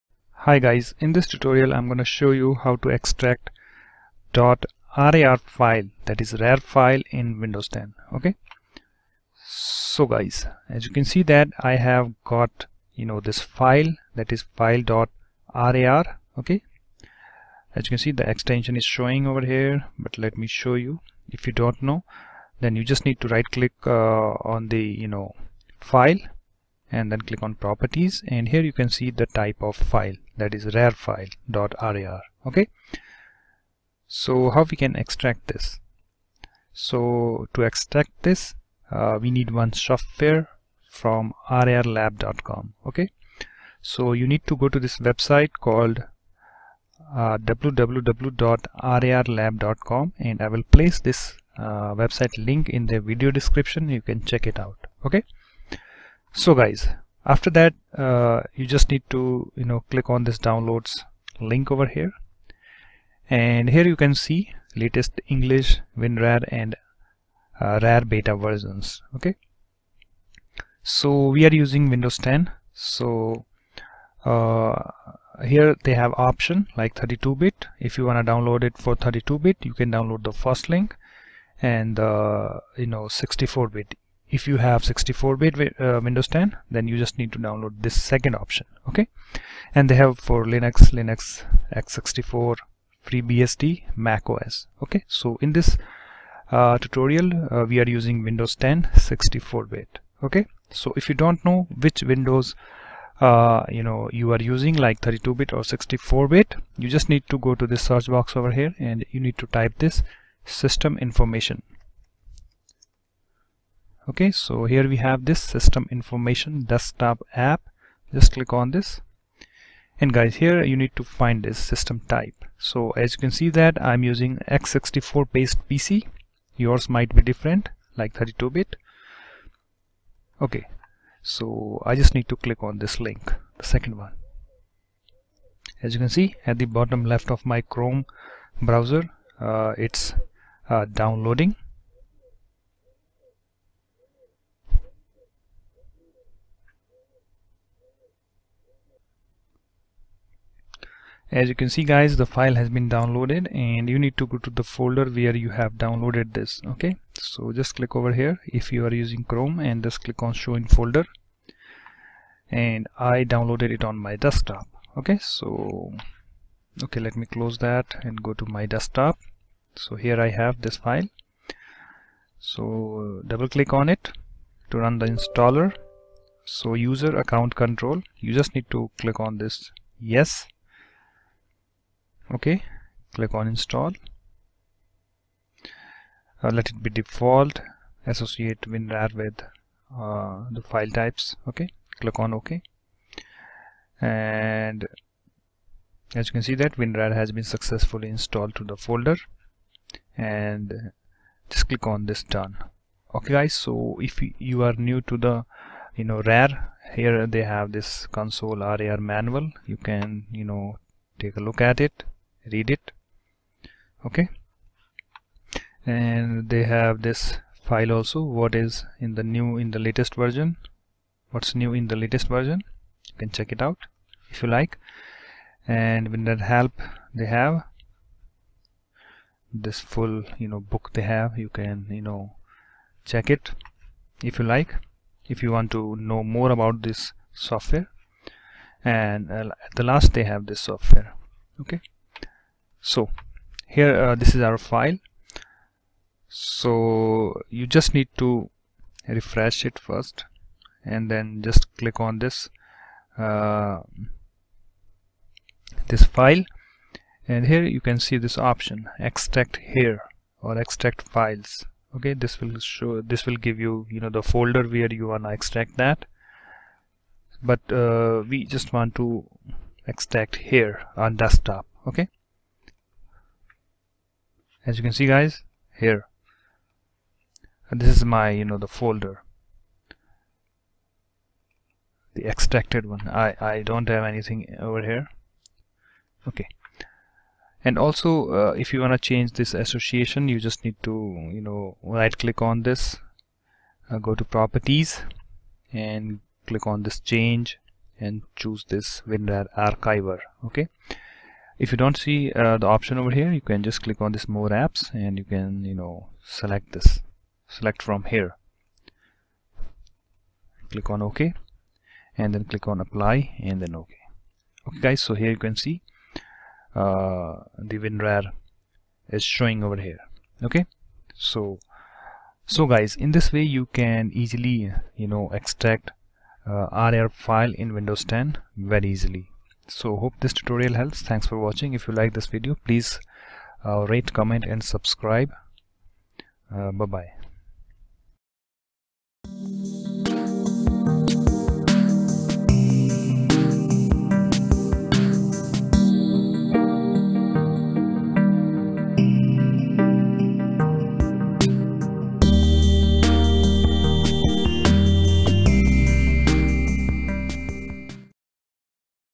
How to Extract RAR File in Windows 10?
Step 2: Click on "Downloads" Link.

Step 3: Click on the version that you want to download like 64 bit or 32 bit.

Step 4: Install the WinRar Software.

Step 5: Go to the file that you want to Extract, Right click on it.

Step 6: Click on "Extract Here".

It's done.